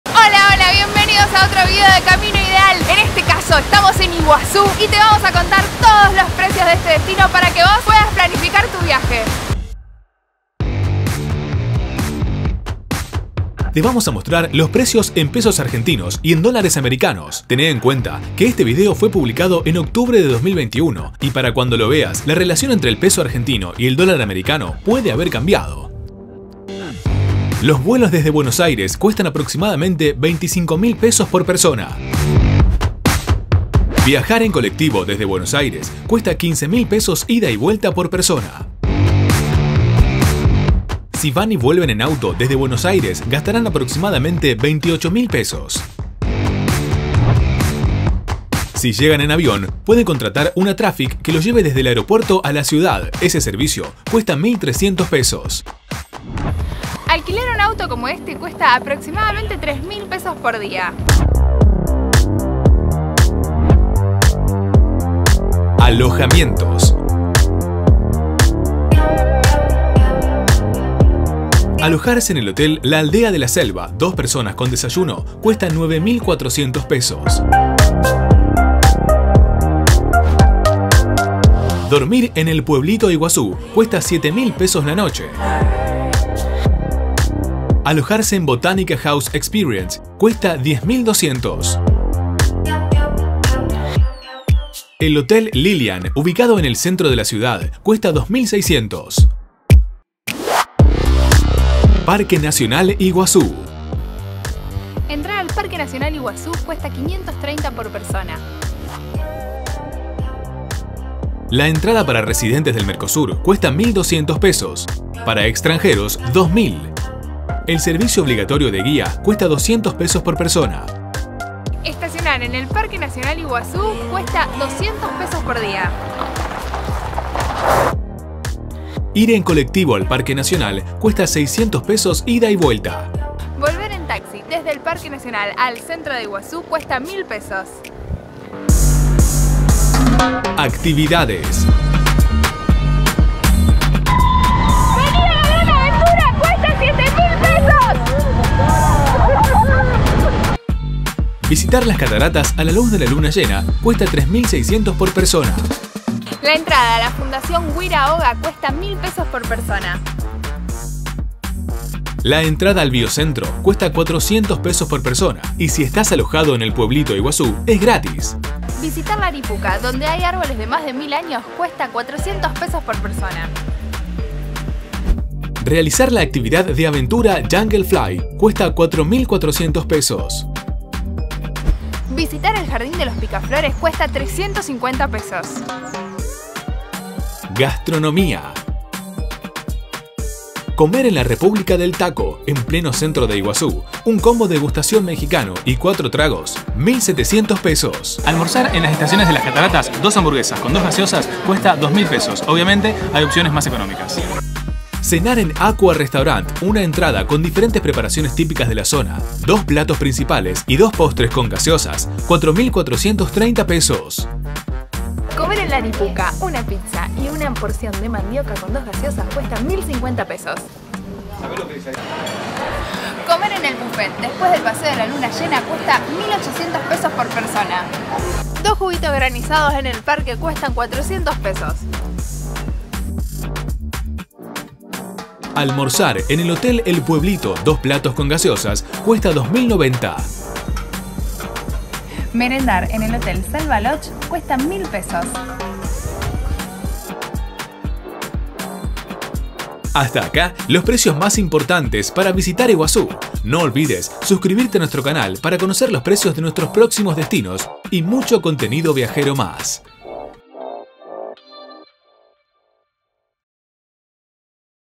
PRECIOS PARA VIAJAR A IGUAZU 🤑 A visitar las Cataratas
Les contamos en detalle los PRECIOS de una de las maravillas del mundo🌎: Las Cataratas del Iguazú. Les traemos TODOS los detalles para que puedan planificar su próximo viaje y organizar su presupuesto 💰 Ya conocen las Cataratas? Tienen ganas de ir?
Una vez que terminen de ver el video, nos cuentan que les parecieron los precios de este Iguazú? Caros o baratos? 😬

00:00 Imágenes Cataratas del Iguazú
00:28 Cómo llegar a Puerto Iguazú precios
00:50 Traslados desde el aeropuerto de Iguazú al centro
00:58 Cuanto cuesta alquilar un auto
01:00 Precio alojamientos
01:45 Precio entrada Parque Nacional Iguazú
02:19 Precio actividades Iguazú
03:07 Cuánto cuesta comer en Iguazú

Más información sobre este y otros destinos de Argentina
Visiten nuestra web👇👇

caminoidealviajes